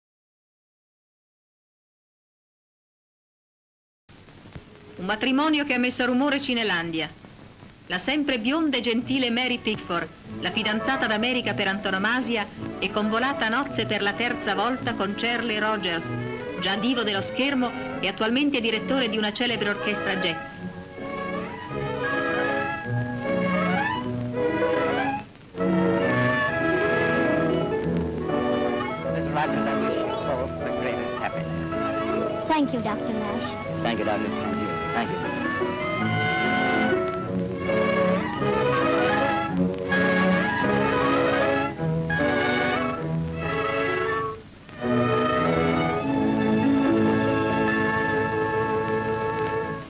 Il terzo matrimonio di Mary Pickford con Charles Rogers
Descrizione sequenze:l'abitazione dell'attrice a Beverly Hills ; fotografi al cancello della villa ; Mary Pickford e suo marito Charles Rogers, direttore di orchestra jazz, nel giardino della villa ; un enorme torta con fiori e nastri su di un tavolino ; la sposa offre una fetta di torta allo sposo ; molte foto per gli sposi ;